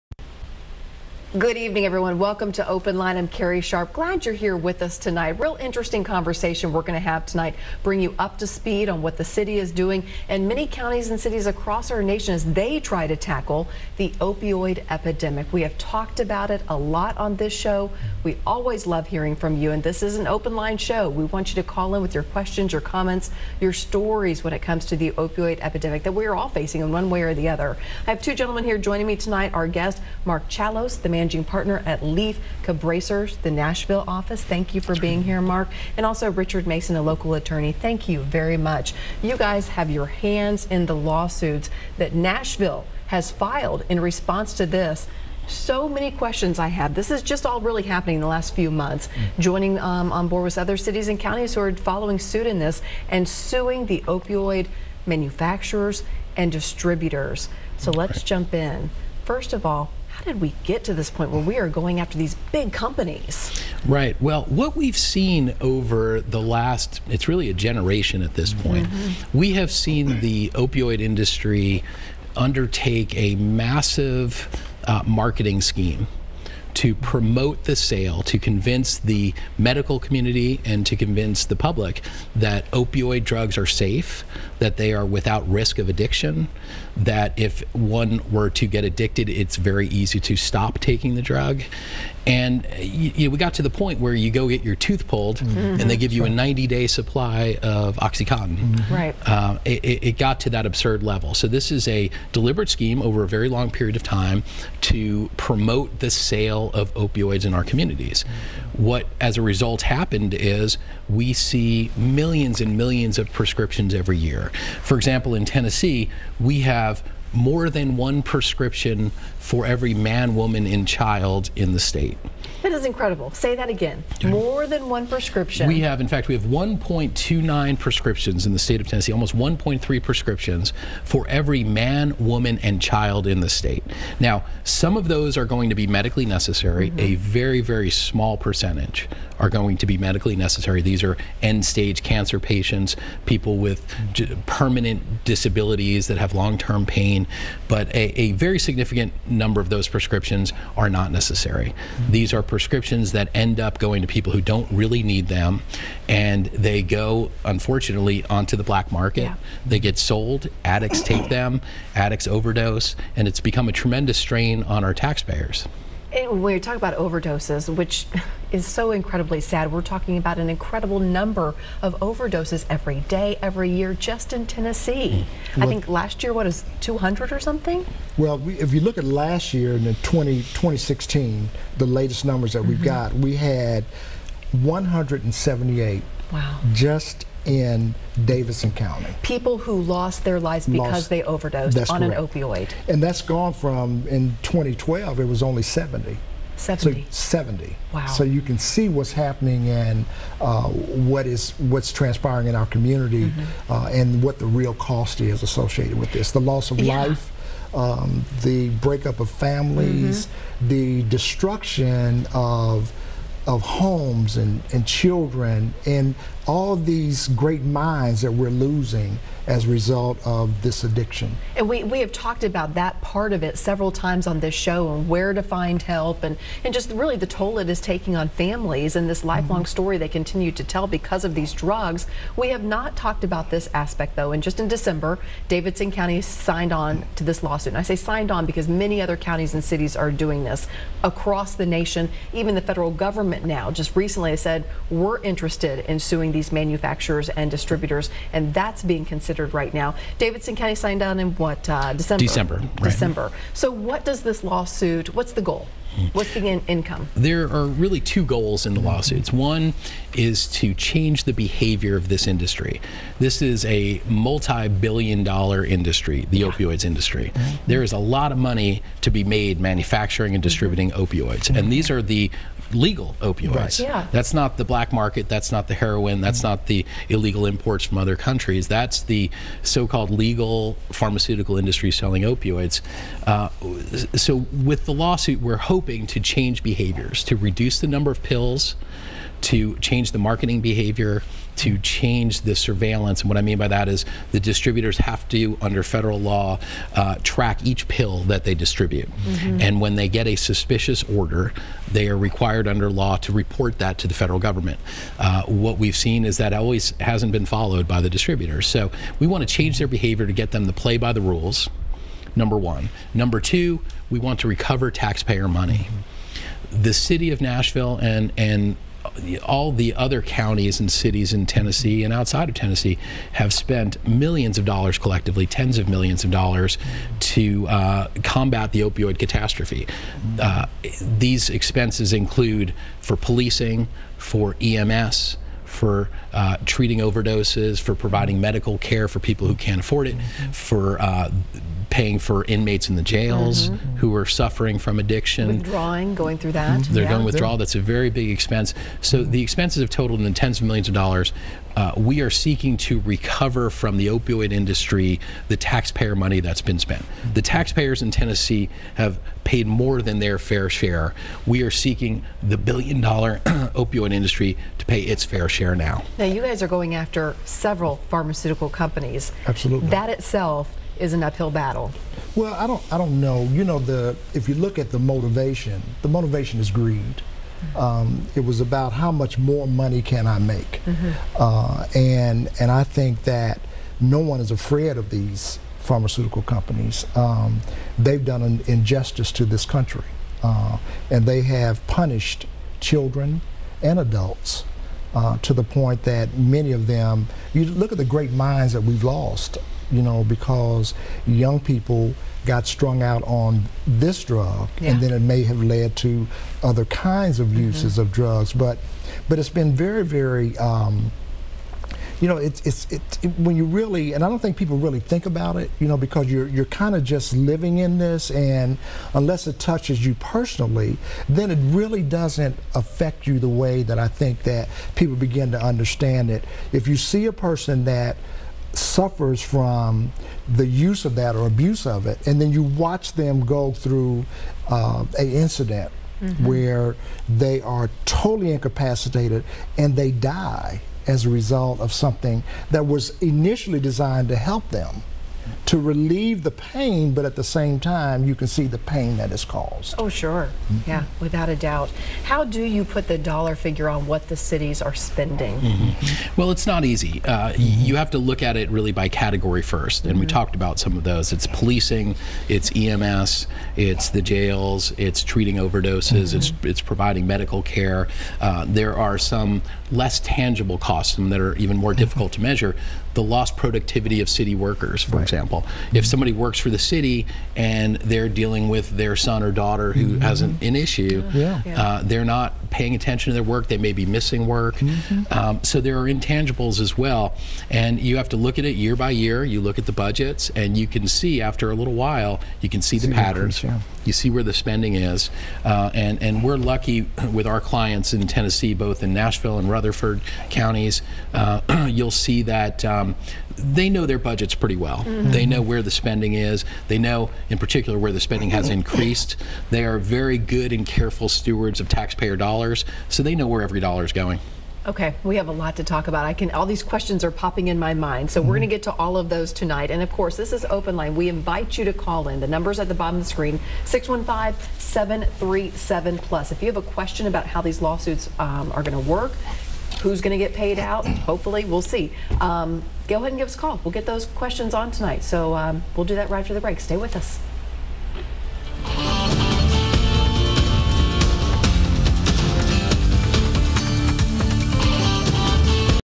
Lawsuit against Opioid Manufacturers p1
The Opioid Crisis, is destroying lives all across Middle Tennessee.  Metro Government has filed a lawsuit seeking justice and restitution for Nashville’s ongoing battle against the opioid crisis that has killed over 200 residents in 2017 alone.  Carrie Sharp NC5 is joined on OpenLine by the top Litigator for this lawsuit, Mark Chalos, and attorney Richard Mason to discuss the need for this case.